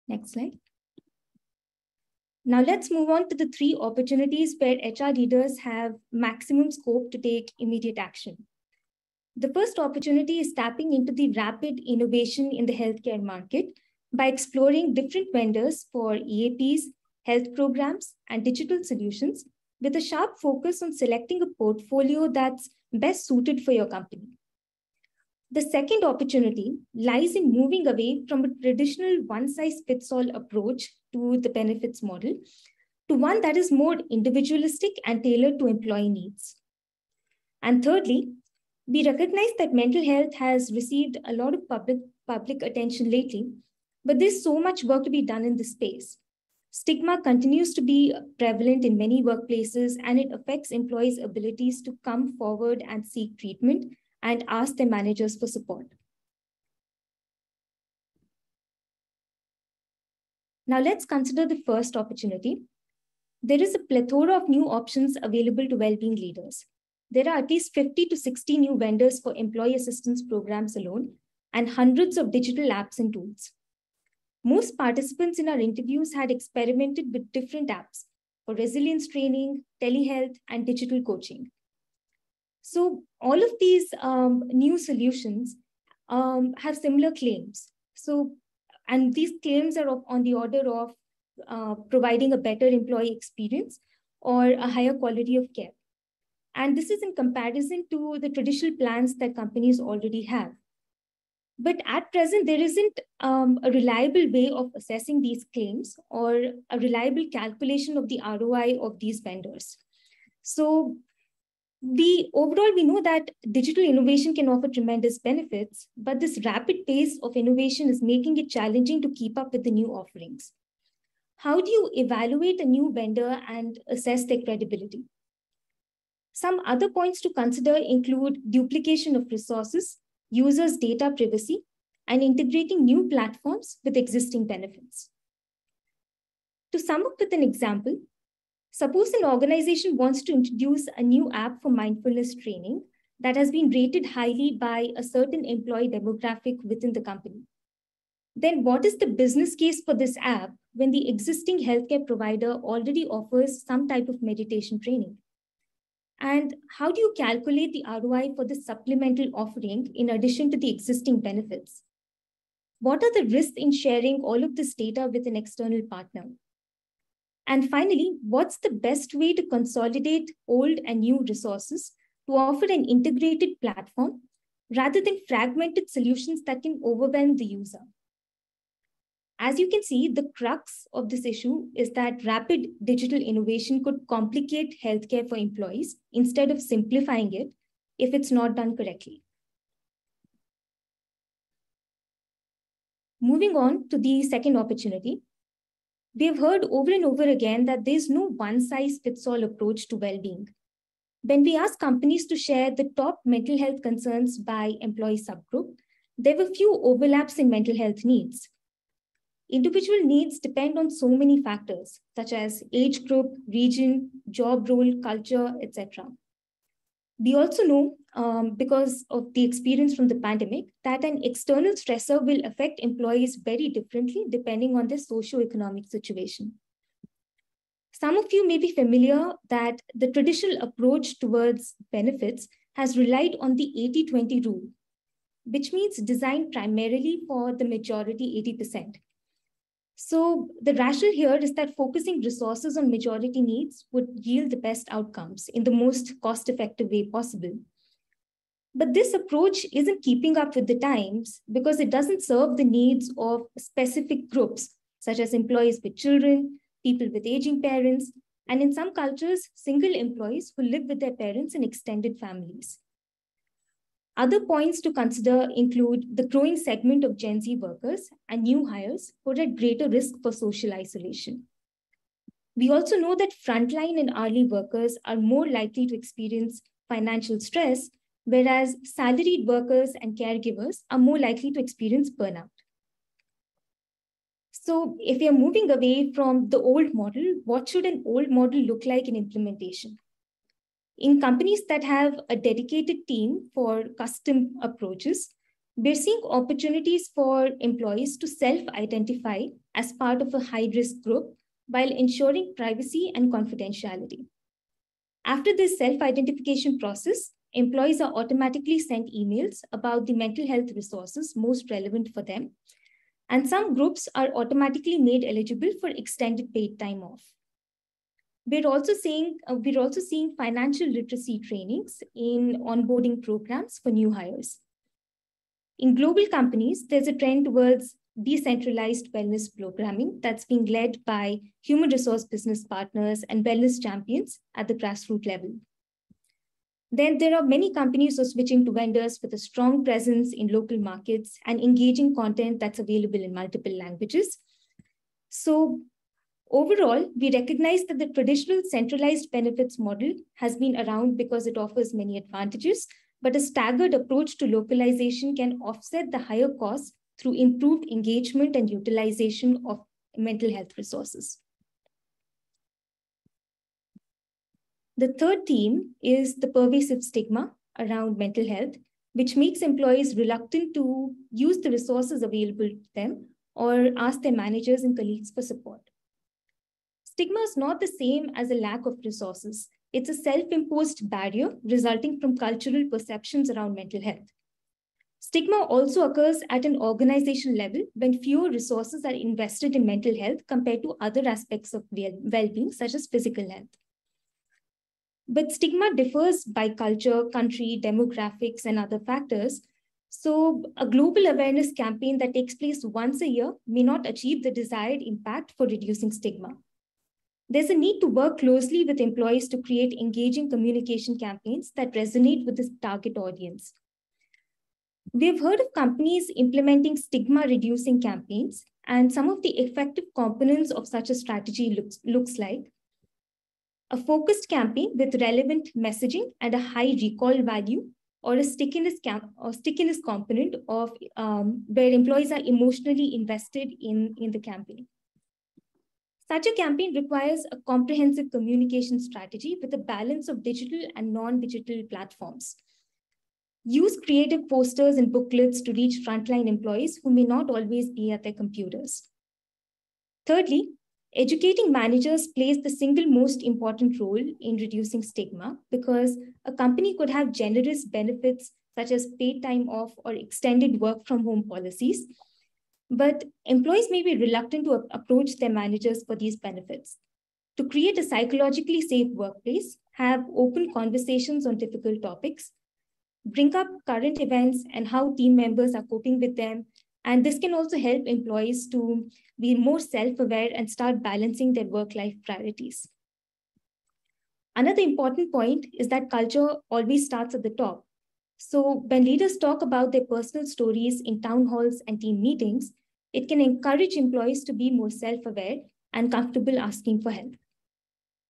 Opportunities for HR leaders to Assist Employees with Well Being
CAHRS RAs discuss three opportunities where HR leaders can take immediate action when it comes to mental health.